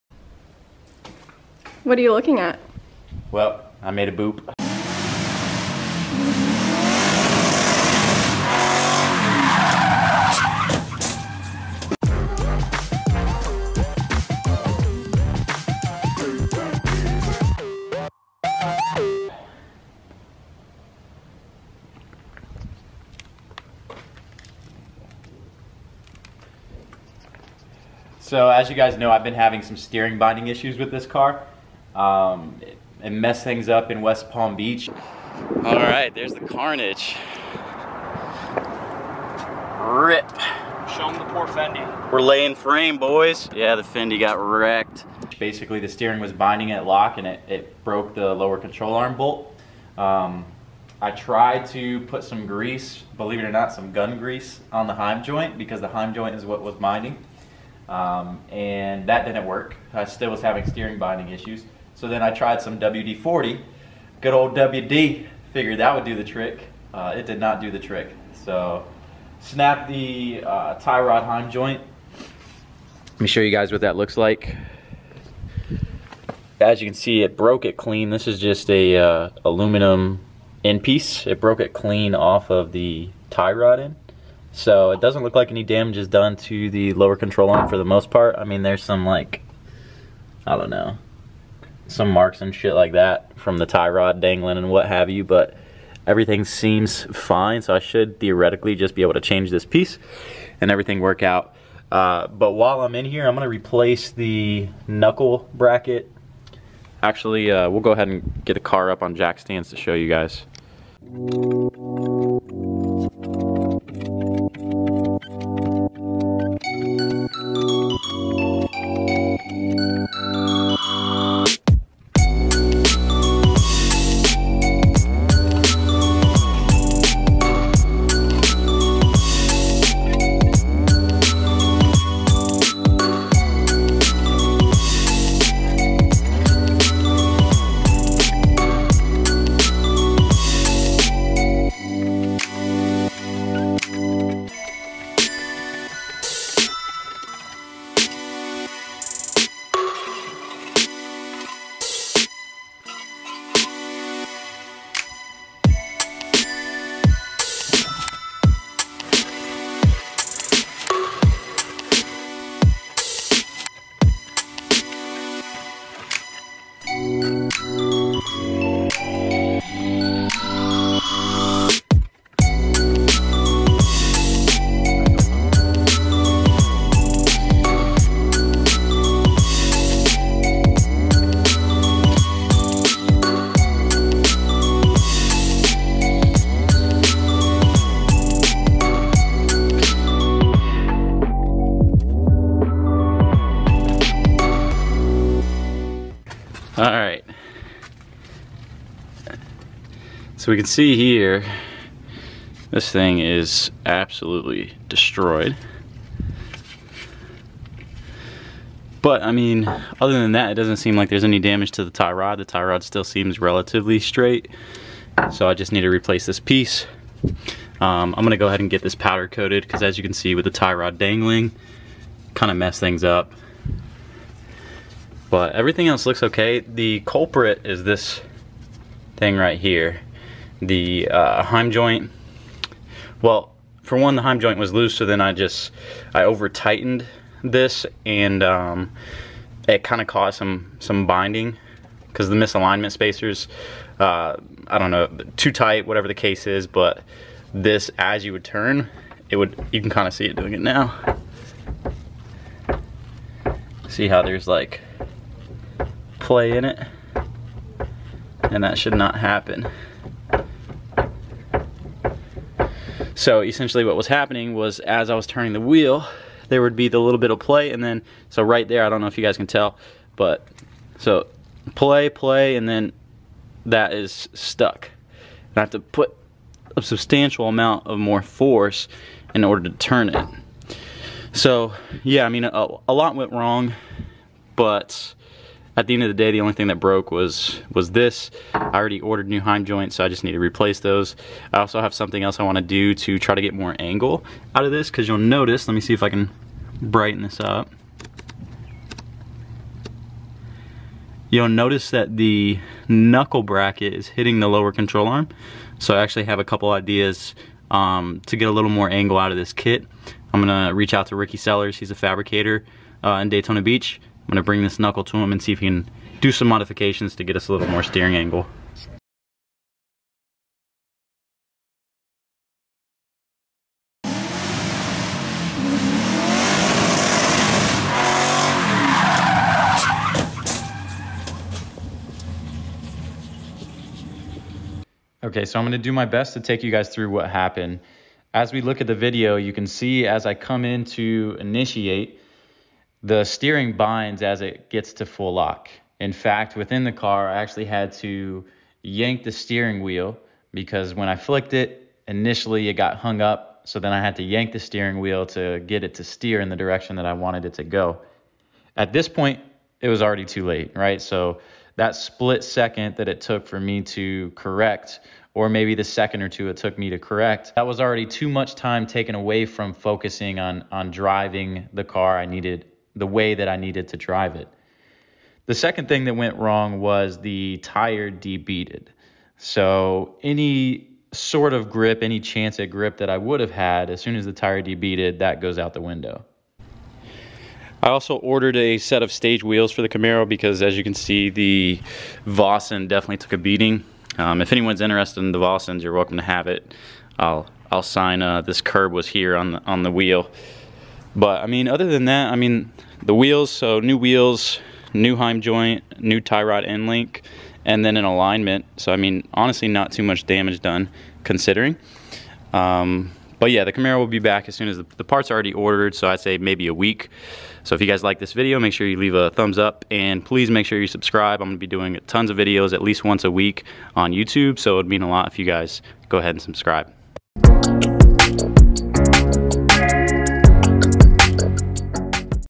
Camaro Crash Drifting Fail
A quick update on the damage to the Camaro. A lot went wrong here but ultimately it was my fault for trying to drive the car (with one hand lol) while the steering components were damaged and I paid the price for that.

Go Pro Interior Mount

Go Pro Steering Wheel Mount

Sparco Racing Gloves

NRG Steering Wheel Short Hub Adapter

NRG Quick Release Kit